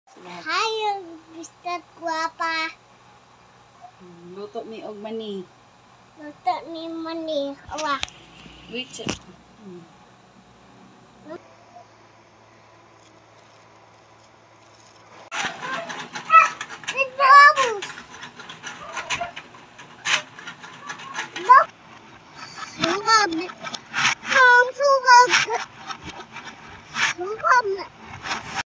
Cooking (Boiling Peanut with salt) with Freya kulot
Ka Pislat Vlogs